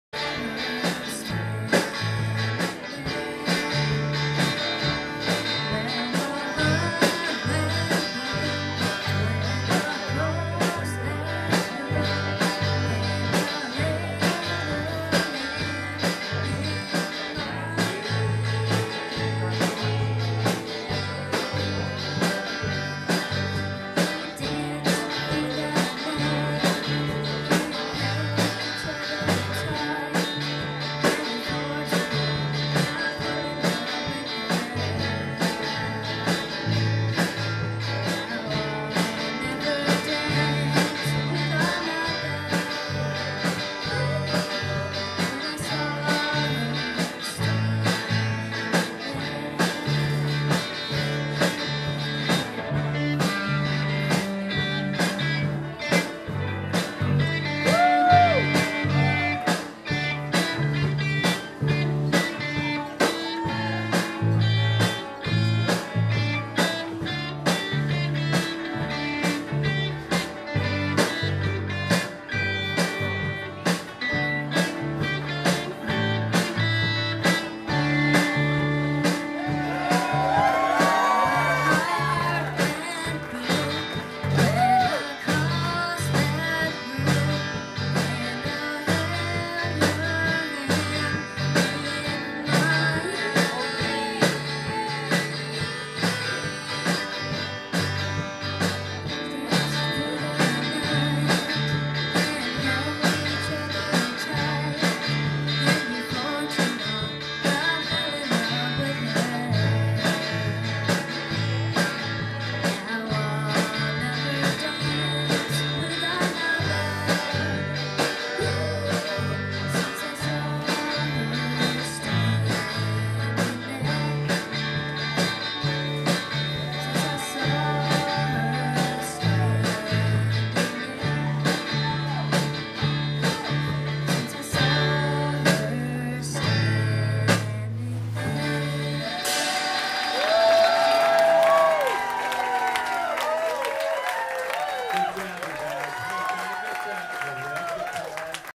Golden Ashes - Song 4
Youth Band Camp, Golden Ashes, presented by Twin Town Guitars at The Whiskey Junction in Minneapolis on June 20, 2014.